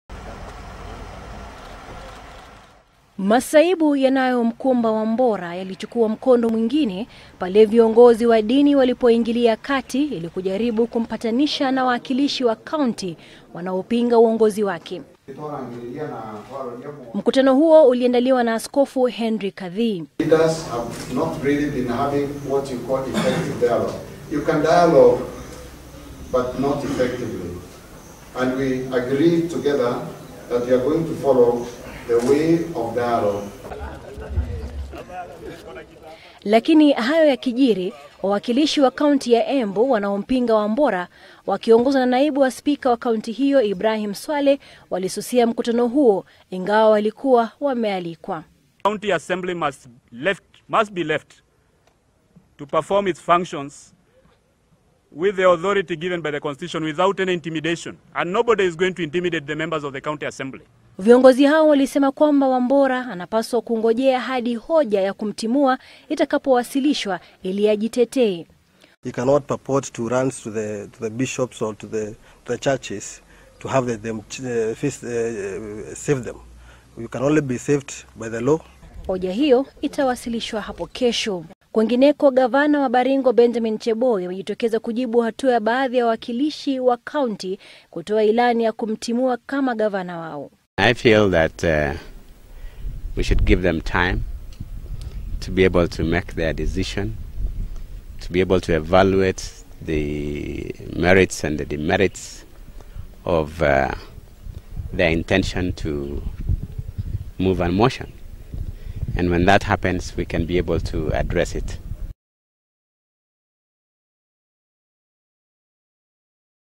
Wambora, Cheboi Matani
Viongozi wa kidini kutoka kaunti ya Kiambu hivi leo waliandaa mkutano na gavana anayekumbwa na utata Martin Wambora na viongozi wengine wakuu wa  eneo hilo, katika juhudi za kutatua mzozo uaoendelea kuhusu njama inayopangwa ya kumtimua Wambora kwa mara ya pili.